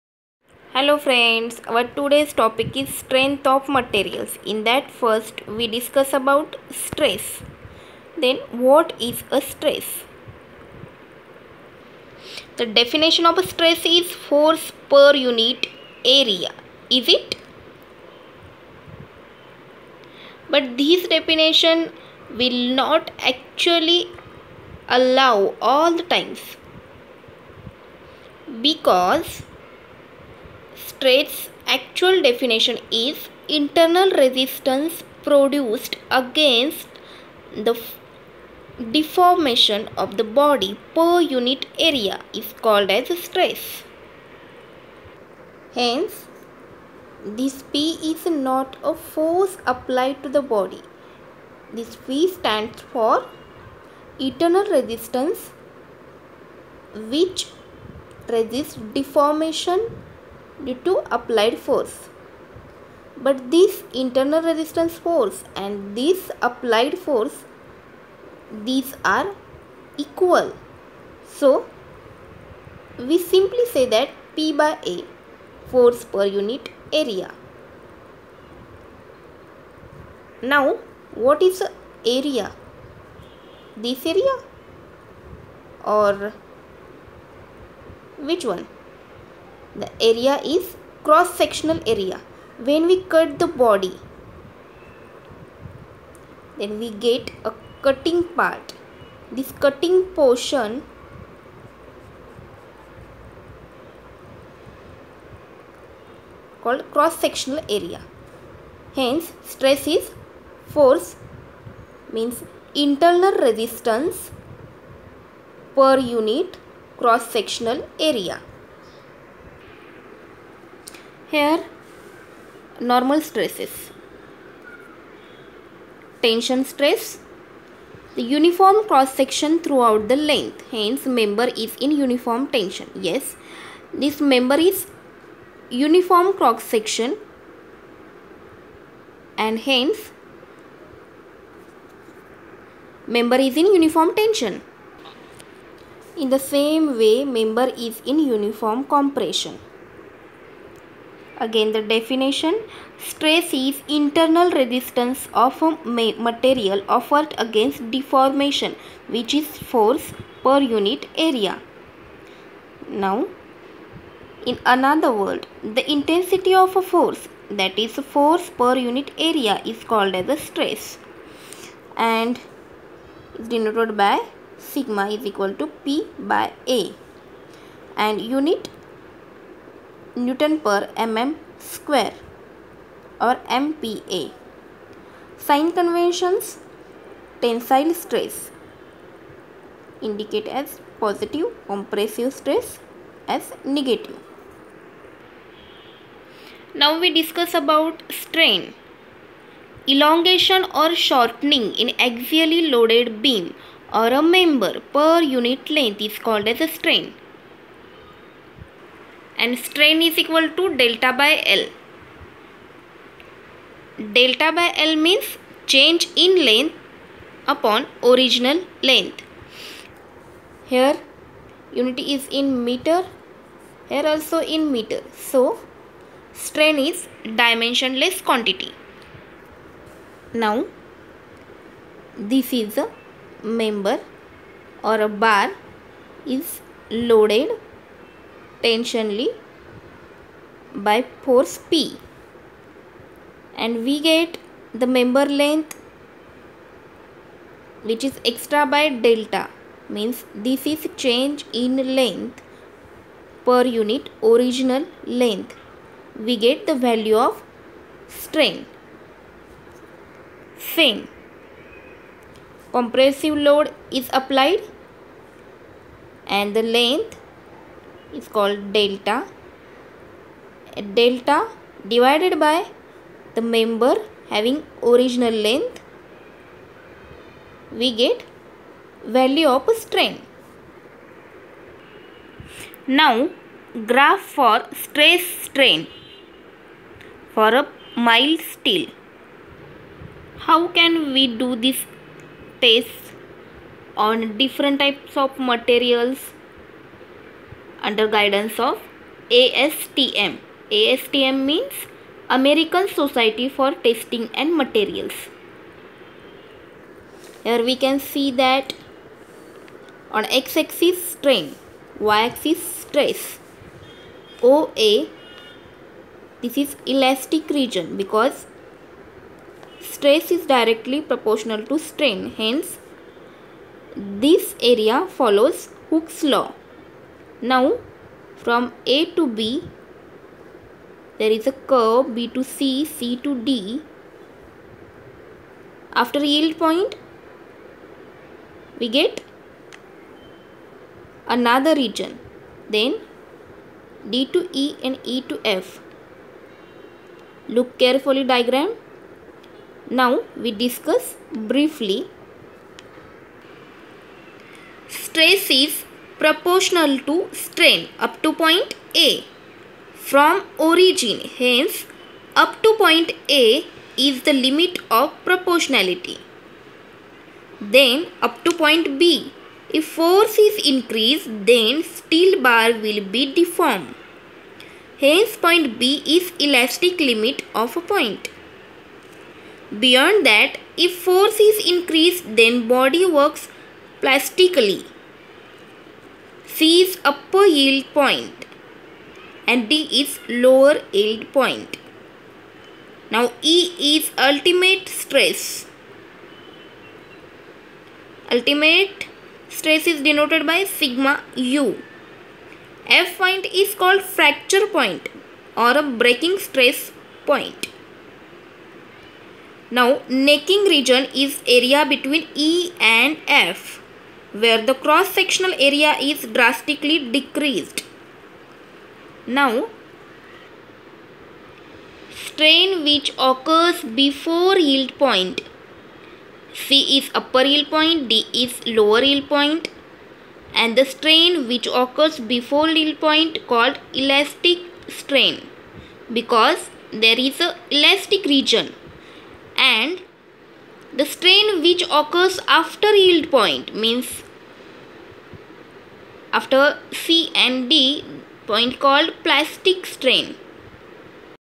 strenth of materials 1
Basic Knowledge about stress and strain.